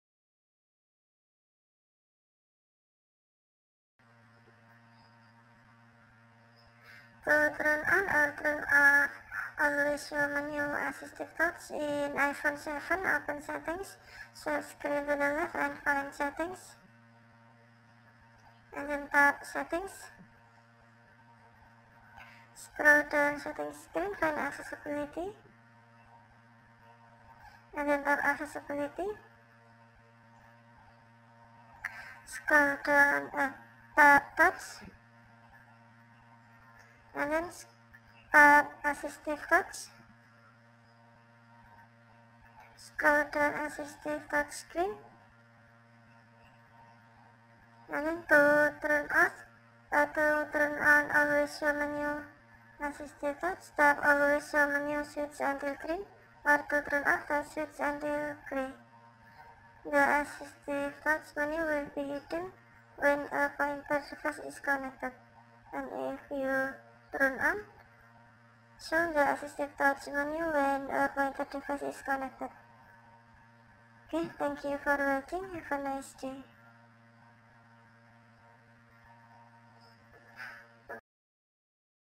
iPhone 7 : How to turn on or off Always Show Menu AssistiveTouch
On this video you can know How to turn on or off Always Show Menu AssistiveTouch in iPhone 7 with iOS 14.0.1 operating system